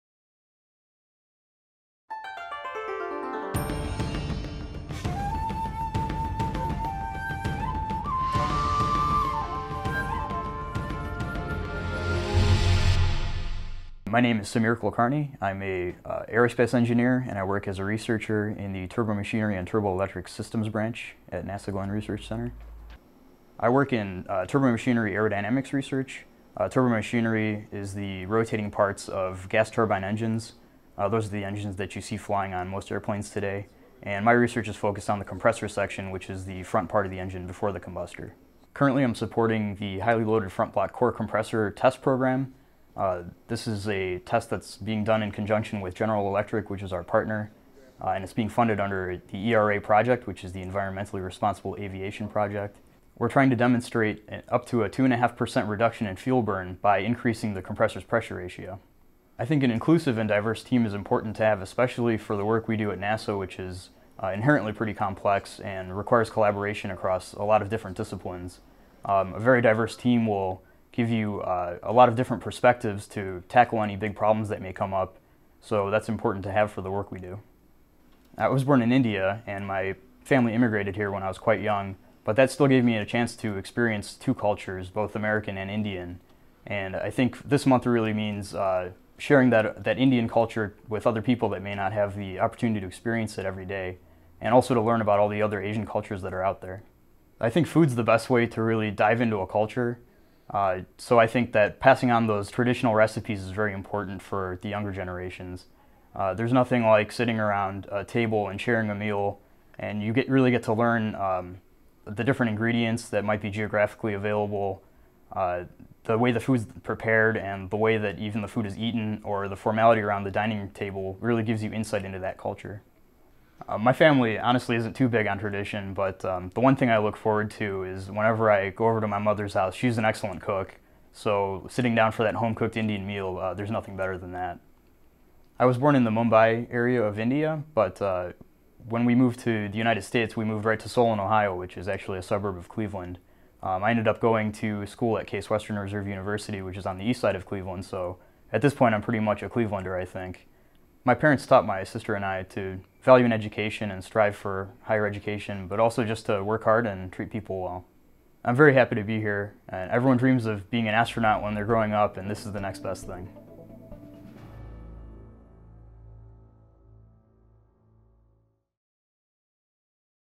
NASA Asian American Pacific Islander History Month - Sameer Kulkarni, Glenn Research Center
Sameer Kulkarni is a research aerospace engineer in the Turbomachinery and Turboelectric Systems Branch at NASA's Glenn Research Center. He currently serve as the research lead for the Highly Loaded Front Block Core Compressor test program in the W-7 test facility (Multistage Compressor Facility) of the Engine Research Building. The effort is a part of NASA’s Environmentally Responsible Aviation Project, with the goal of demonstrating compressor technologies which increase fuel efficiency of modern turbofan engines. Kulkarni works with industry partners and NASA engineers to define the test requirements and execute the test plan. He uses computational fluid dynamics simulations and test data to enhance current understanding of compressor aerothermodynamics.